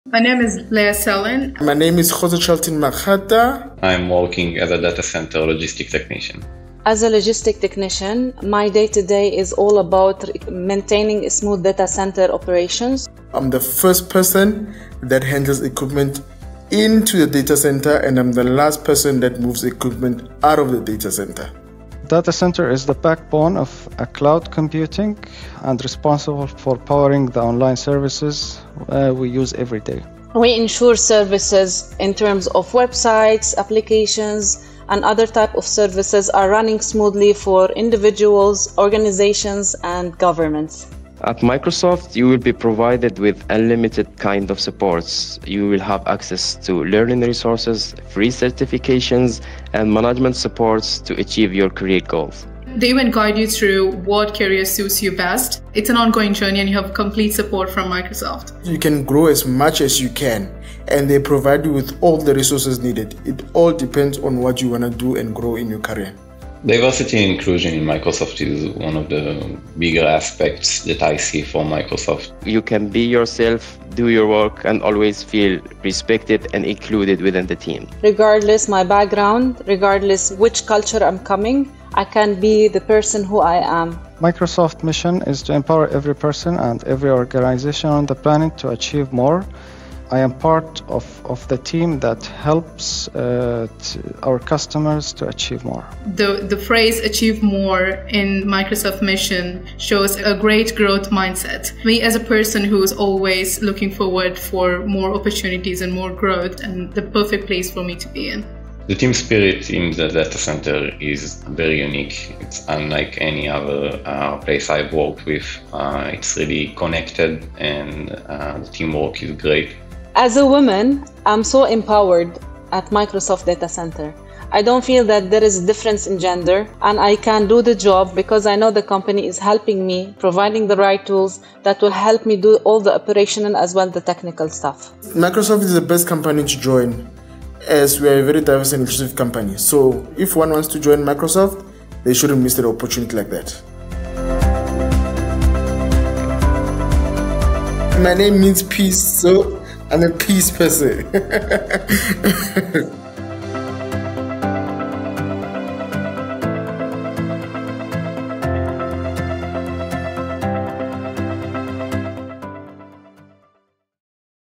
Join us as a Datacenter Logistics Technician
Stretch your skills—and make a difference while you do it—as a Microsoft Datacenter Logistics Technician.

Keeping billions of connected devices and systems working for millions of people offers more than purposeful steady work with great benefits. It could be the springboard for a lasting, fulfilling career in tech.

We teach you everything to get started. And once you’re up and running, there’s on-the-job training with the latest technologies to help further your growth.